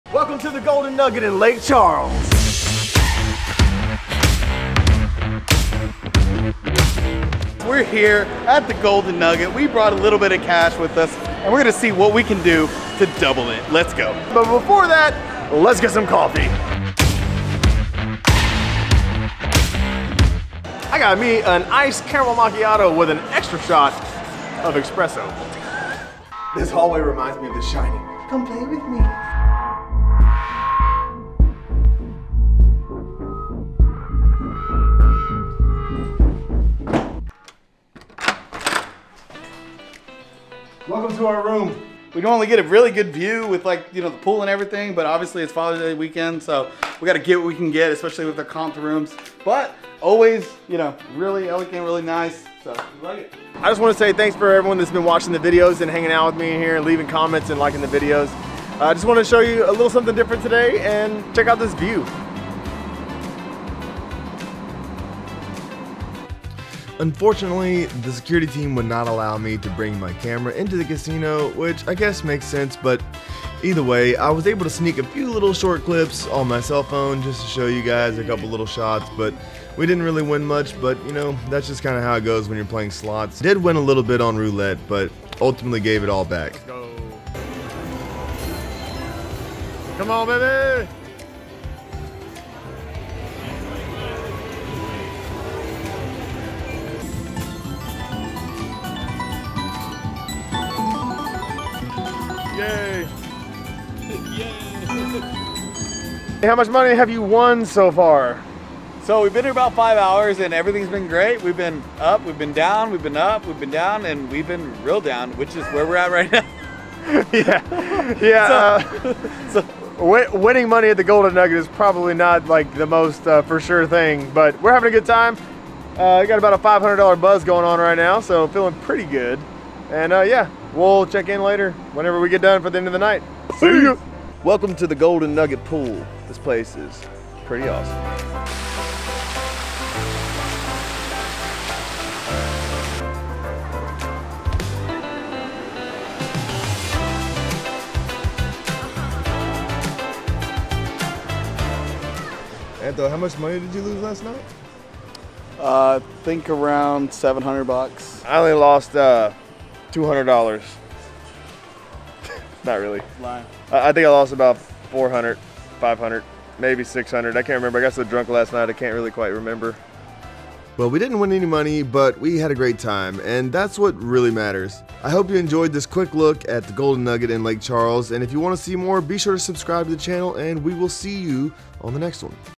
Day Trip to the Golden Nugget in Lake Charles!
Going to the Casino is always a fun time, as long as you go with the mindset of losing money and only taking what you can afford to lose. The Golden Nugget in Lake Charles is a very nice place with tons of fun things to do and some amazing restaurants.

Thanks for everyone that sticks around for the end of the videos.

Lens: Sigma 24-70 f2.8